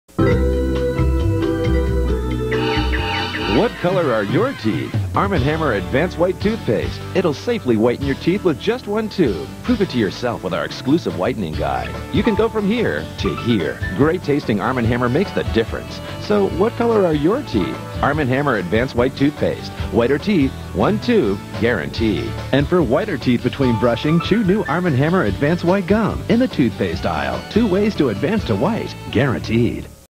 Arm & Hammer | Advance White Toothpaste | Television Commercial | 2001
Arm & Hammer is a registered trademark of Church & Dwight, a major American manufacturer of household products. The logo of the brand depicts the ancient symbol of a muscular arm holding a hammer inside a red circle with the brand name and slogan. Originally associated solely with baking soda and washing soda, the company began to expand the brand to other products in the 1970s by using baking soda as a deodorizing ingredient. The new products included toothpaste, laundry detergent, underarm deodorant, and cat litter.